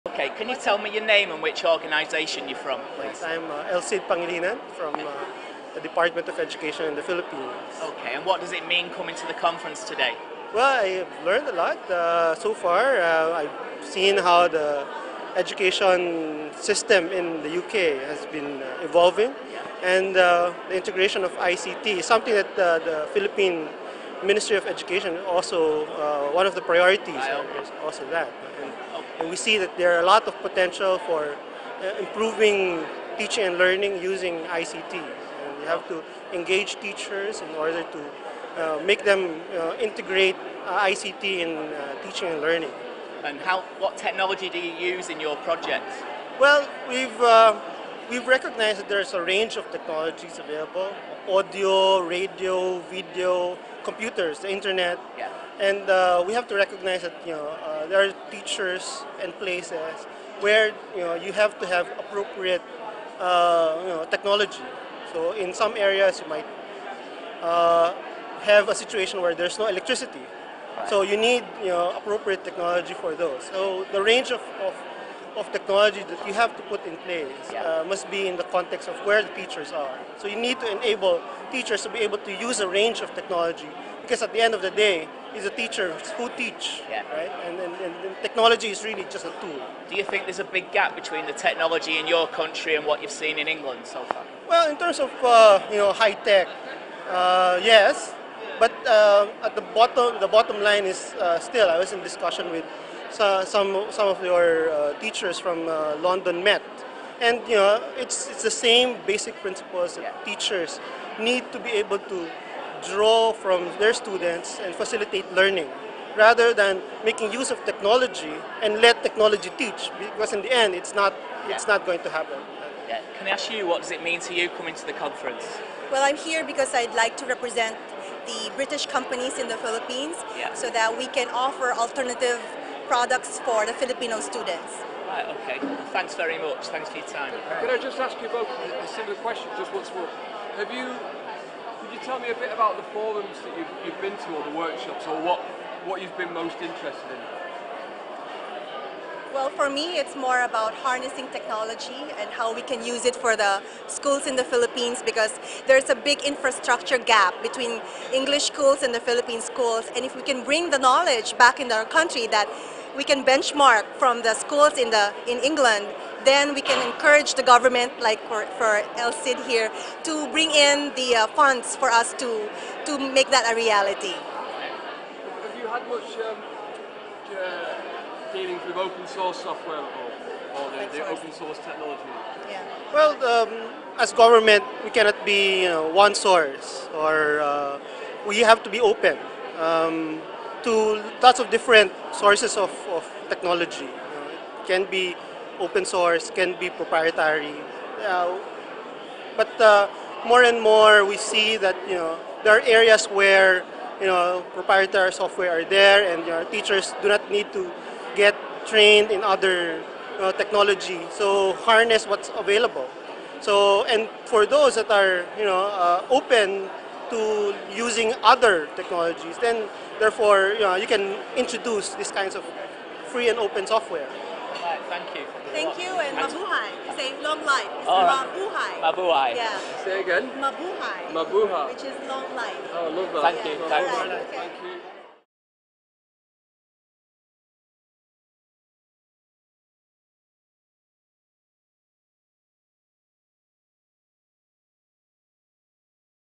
Department of Education Philippines
Two members of the Department of Education Philippines tell us about how the techology that is used for education has to be appropriate to the situation of the schools and teachers. What about when there is no electricity in the schools?